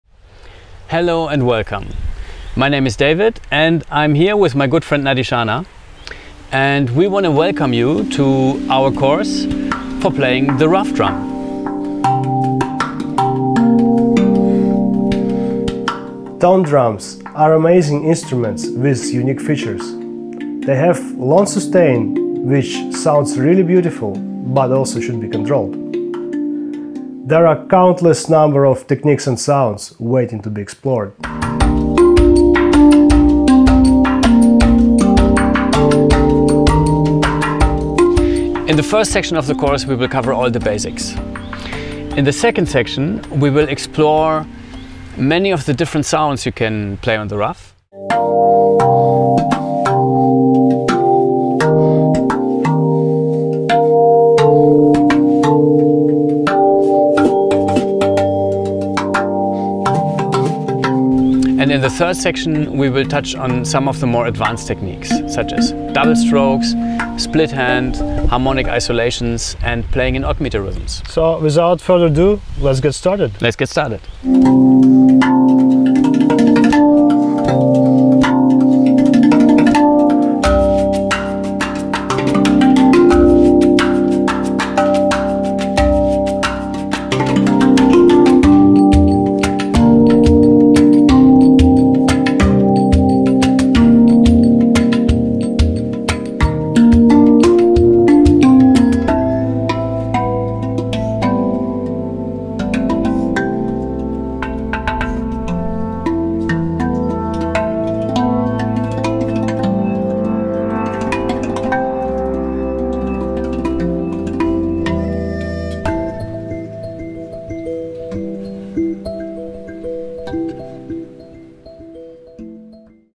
RAV Vast & Tongue Drums
Do you have a RAV or tongue drum or are interested to add a new unique sound to your repertoire?

▸ How is playing this instrument different from playing handpan?
▸ How to use and control the long sustain?
▸ Which are the sounds waiting to be discovered below the surface?
▸ Nadishana and David K take you on a  journey to discover the unique world of tongue drums.